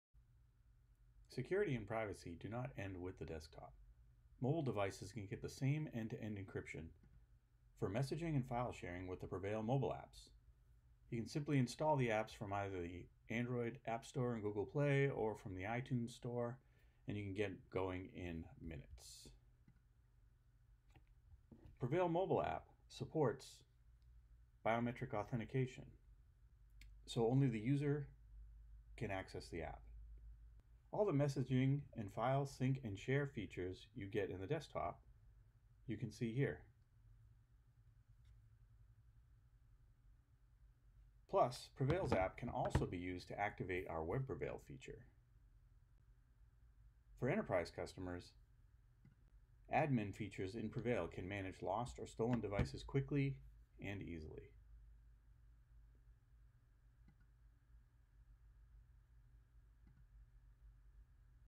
Mobile (product video)
This is a product video detailing PreVeil's functionality on mobile platforms.